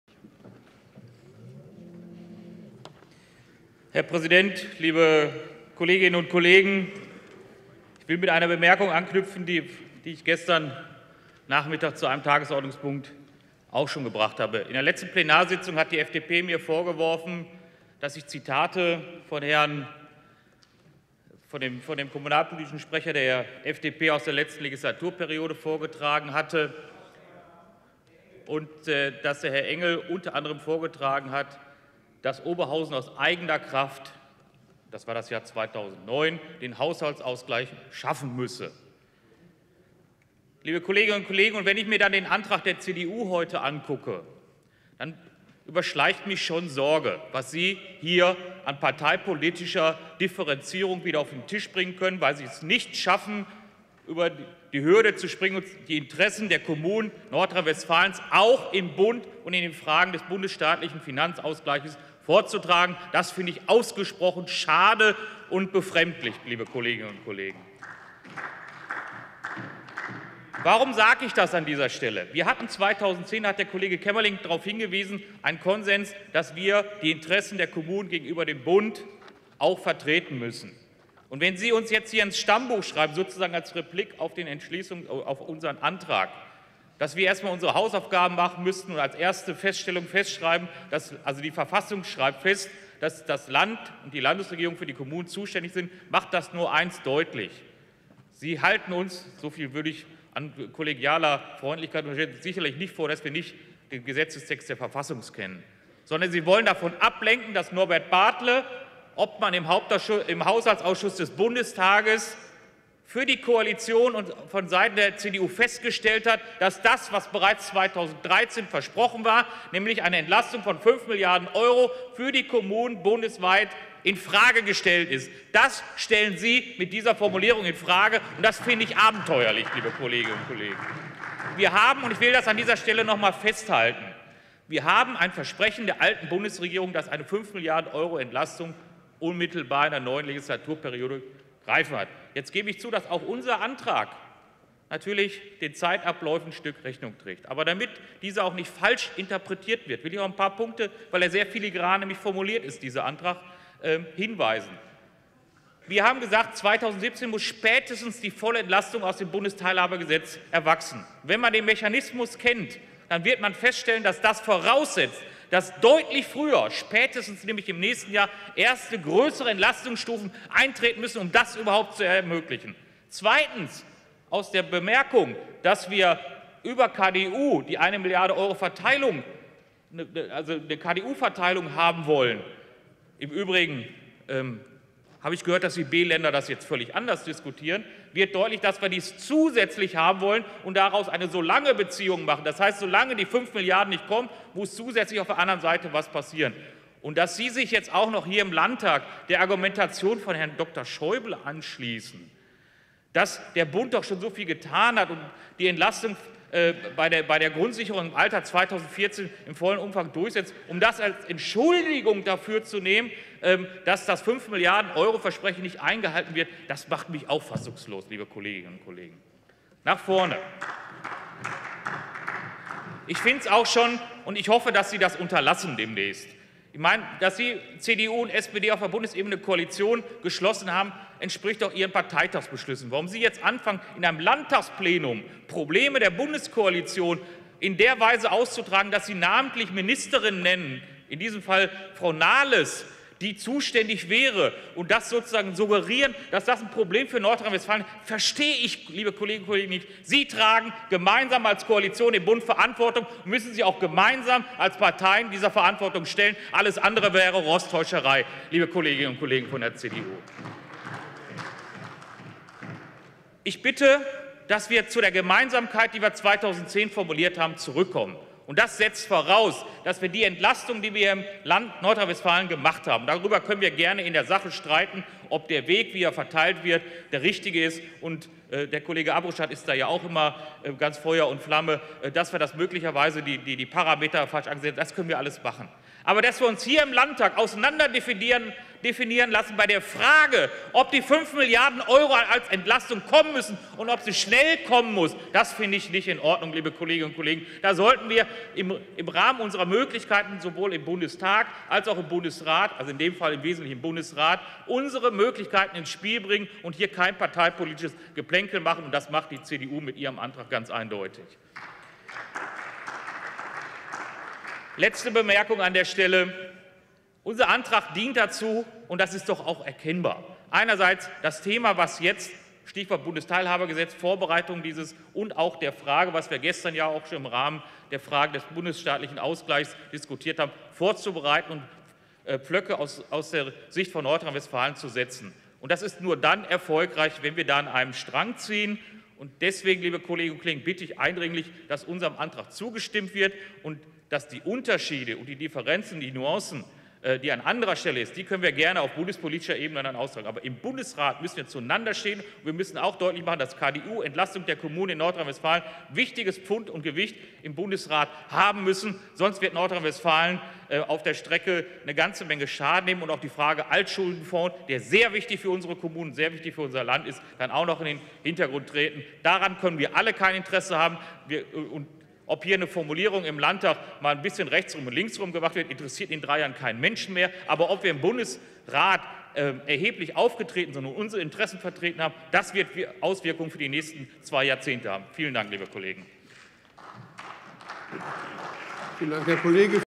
Plenarrede von Mehrdad Mostofizadeh zum Thema finanzielle Entlastung von Kommunen
Am 10. April 2014 hielt der Grüne Abgeordnete und finanzpolitische Sprecher Mehrdad Mostofizadeh im Landtag NRW eine Rede zum rot-grünen Antrag "Gemeinsame Kraftanstrengung von Bund, Land und Kommunen fortführen -- Finanzielle Entlastung von Städten, Gemeinden und Kreisen im Zuge des angekündigten Bundesteilhabegesetzes sicherstellen"